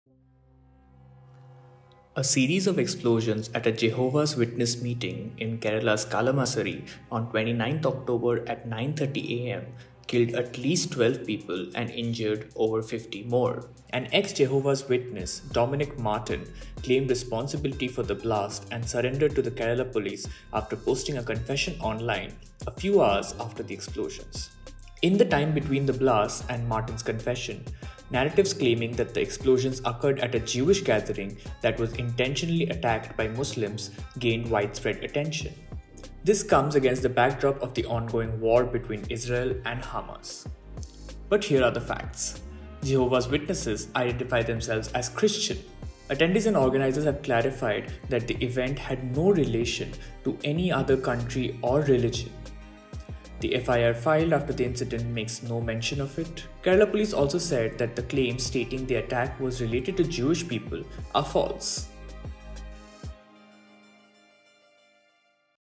Kerala Serial Blasts | Blasts targeting Jehovah’s Witnesses falsely linked to Israel-Hamas conflict
Many social media accounts have falsely claimed that the blasts in Kerala targeted the Jewish community. Here’s the truth. Fact-check by Soham Shah.